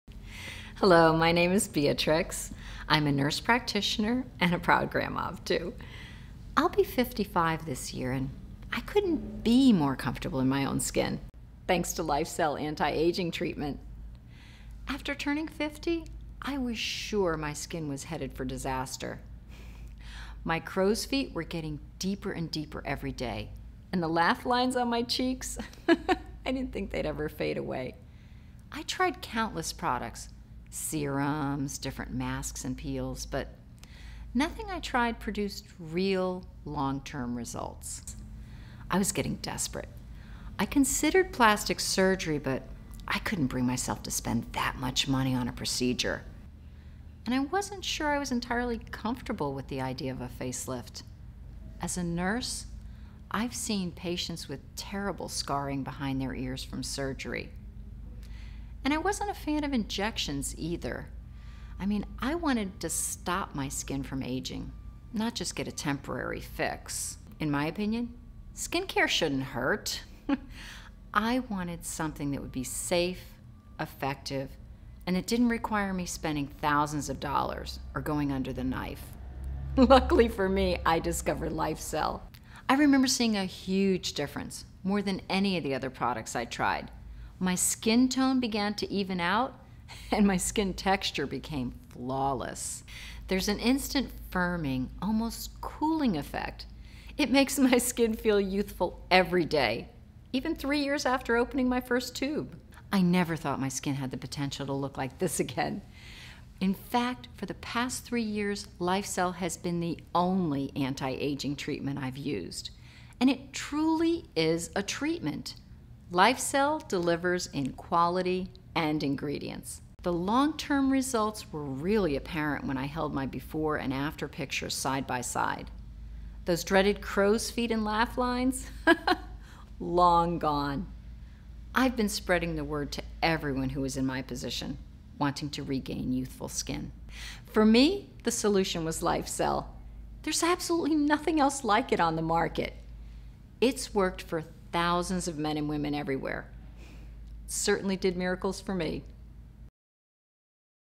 Beatrix Talked About Her LifeCell Anti-aging Treatment Experience
Beatrix, turning 55, a nurse practitioner and a proud grandma of 2 shared her experiences with aging skin. She had a lot of skin issues like crow's feet and laugh lines on cheeks. And even considered those expensive plastic surgery options but when she discovered LifeCell anti-aging treatment, she had no regrets upon seeing her before and after photos.Now, She's a happy grandma turning 55 this year that was able to achieve a youthful looking skin.

"LifeCell Anti-aging treatment makes my skin feel youthful everyday". "For the past 3 years, LifeCell is the only anti-aging solution I've used" - Beatrix

Try LifeCell Anti-aging Treatment Today for 30 days "Youthful skin at any age"